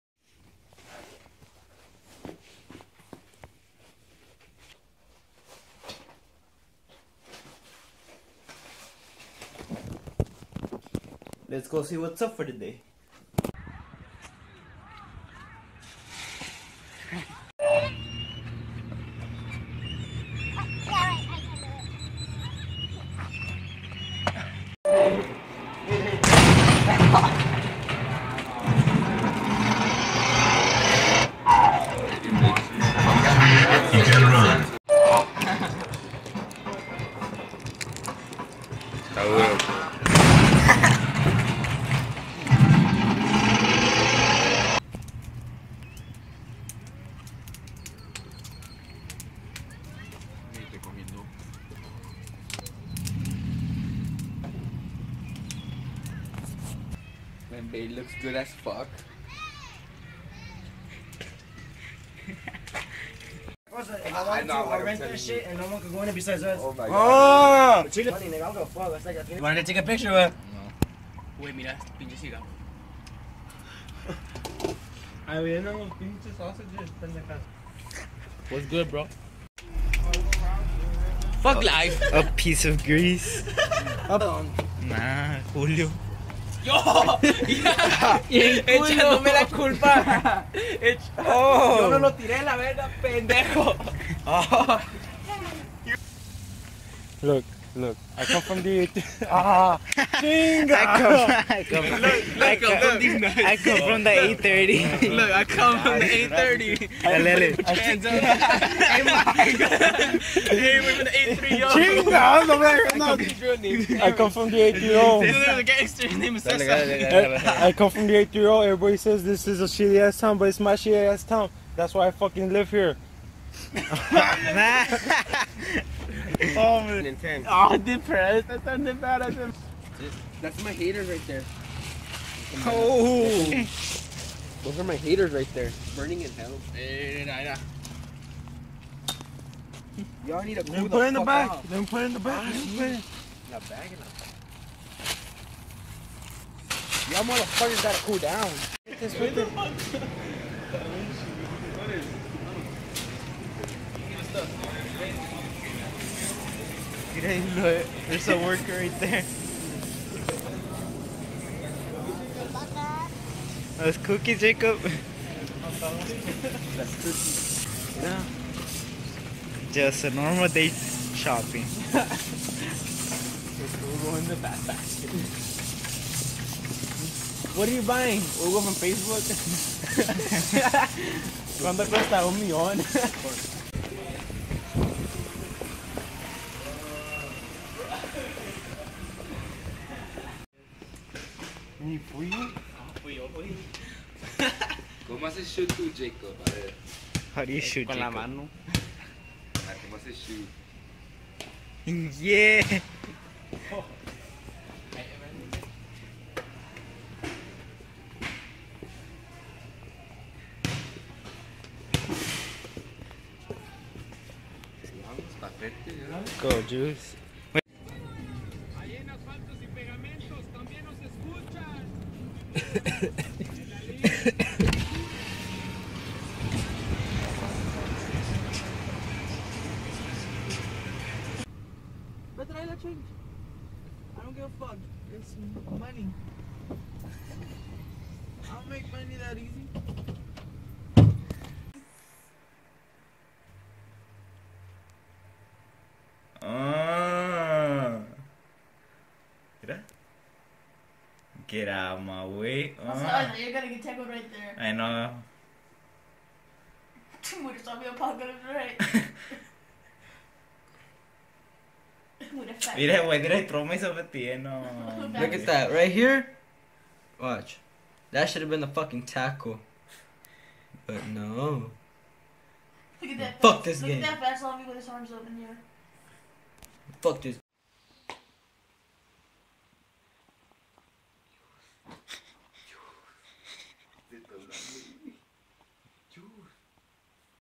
VlogEpisode4: Chilling Killing
Just hanging out with the crew everyday on spring break haha😂 and I will try to vlog more I really enjoy vlogs. I appreciate those who been sticking with me!